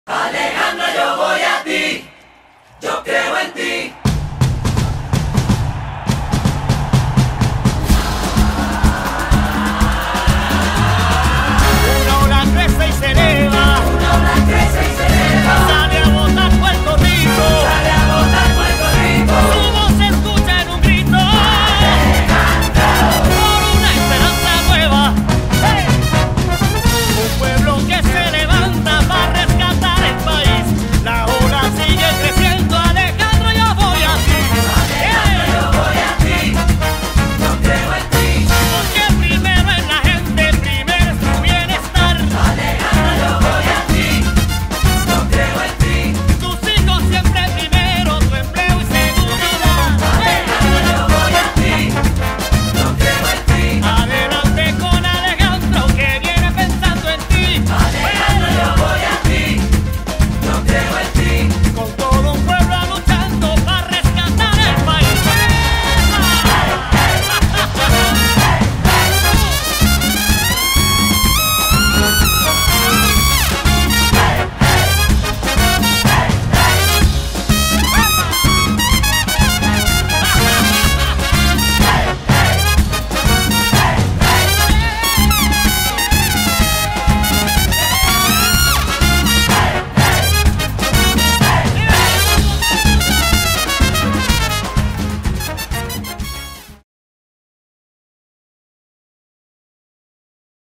Rally Ola de la Gente Alejandro Garcia Padilla 2012
ALejandro Garcia Padilla en la actividad llamada La Ola de la Gente Cabo Rojo, San German, Hormigueros, Mayaguez, Anasco ESTO FUE ELABORADO POR LA COMPANIA PJ FILMS WILBERTO RUIZ DIRECTOR